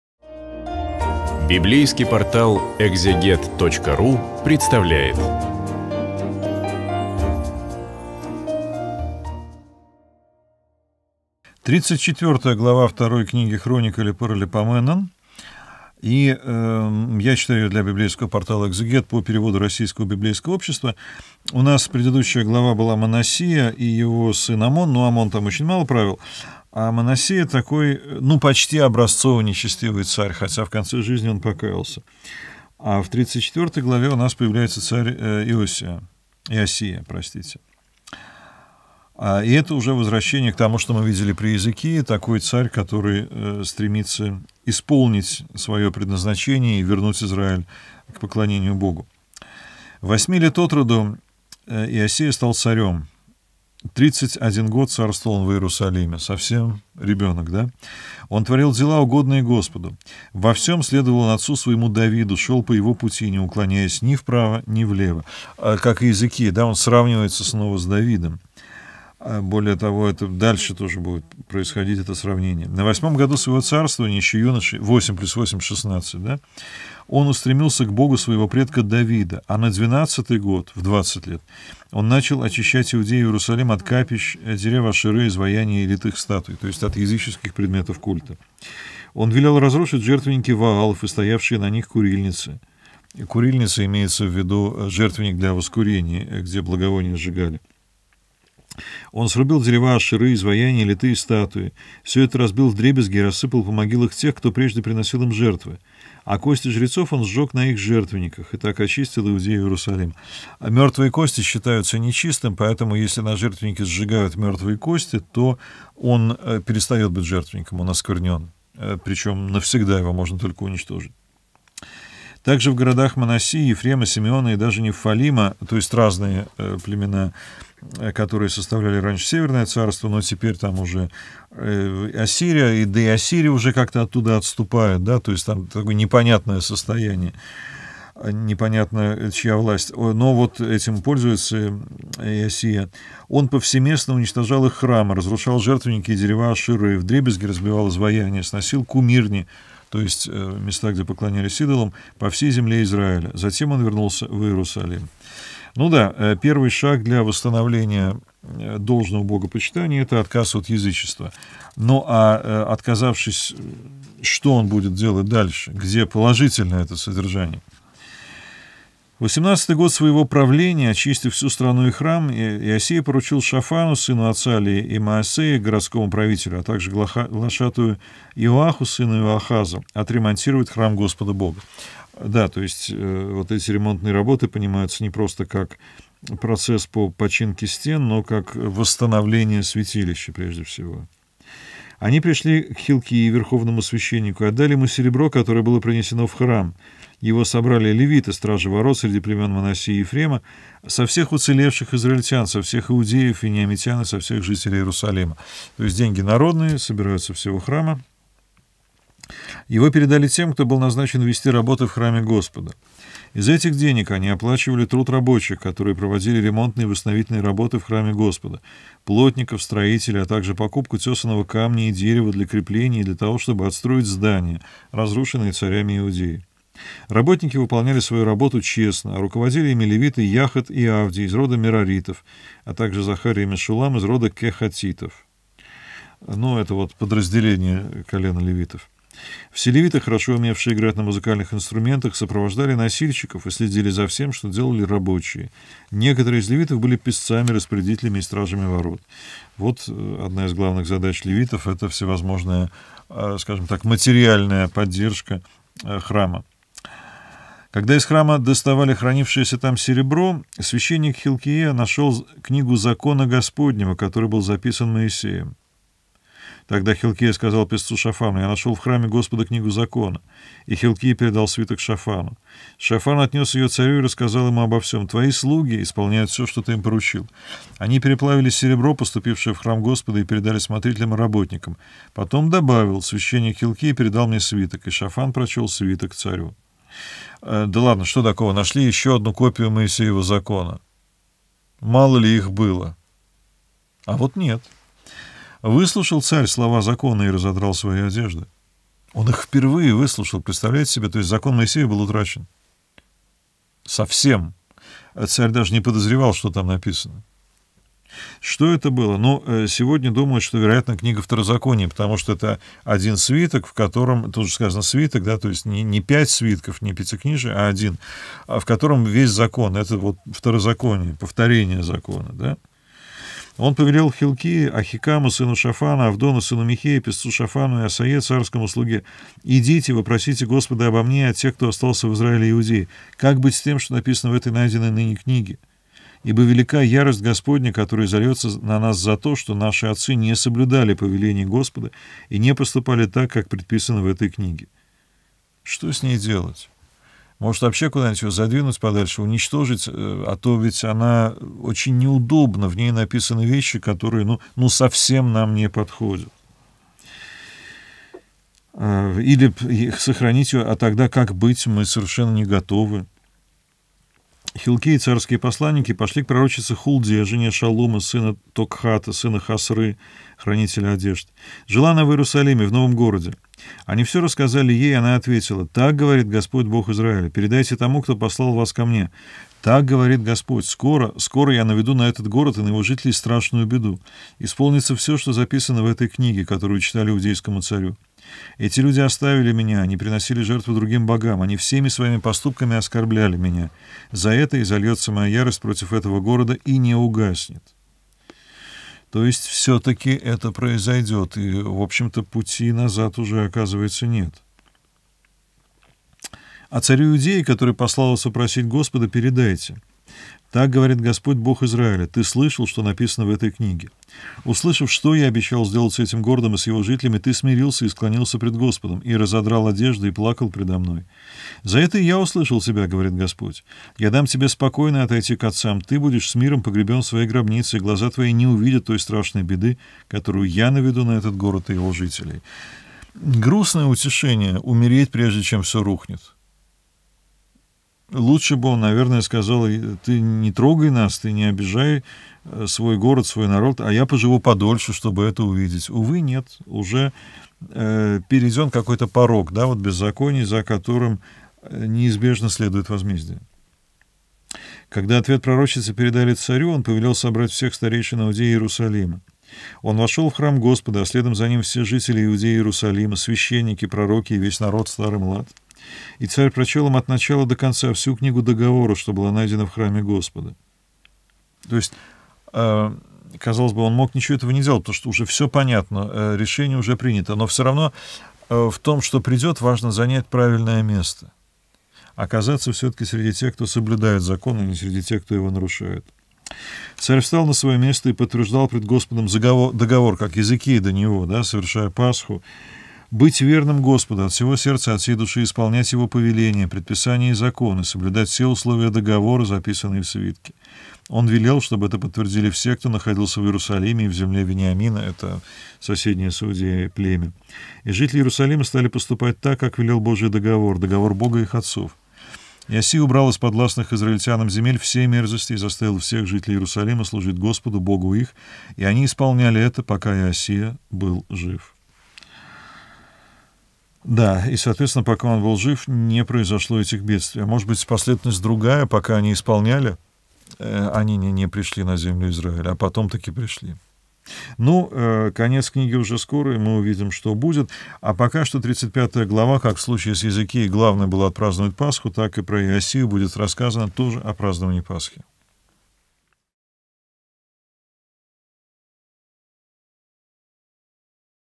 2-я Книга Паралипоменон. Глава 34. Андрей Десницкий. Ветхий Завет
В 34-й главе мы читаем про Иосию, который воцарился в возрасте восьми лет и царствовал 31 год. Правление его было благочестивым, и он напоминал царя Езекию. Хронист сравнивает его с царем Давидом. Он, как и его предок Езекия, уничтожал языческие капища и разрушал жертвенники Ваала. Он также очищал храм и возобновил богослужения.